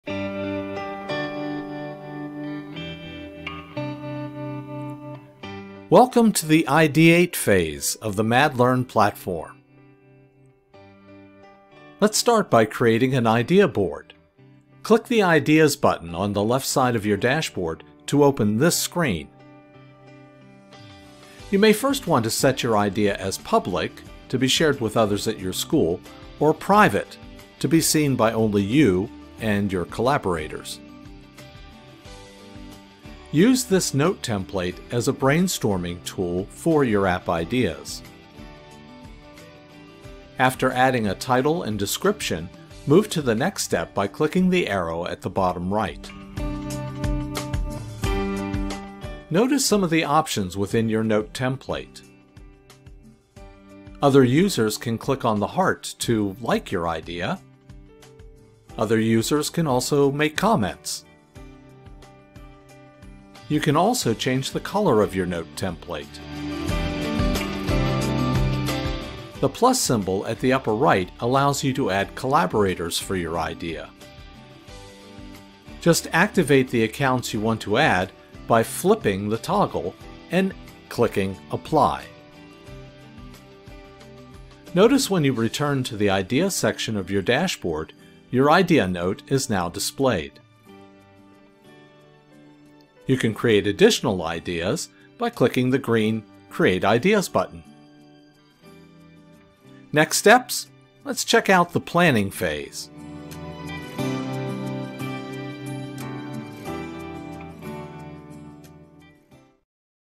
MAD-learn Ideate Phase
The revamped MAD-learn platform takes students through a step-by-step journey of the engineering design process. In the ideate phase, students are able to brainstorm their amazing app ideas and decide which ones they are most passionate about building.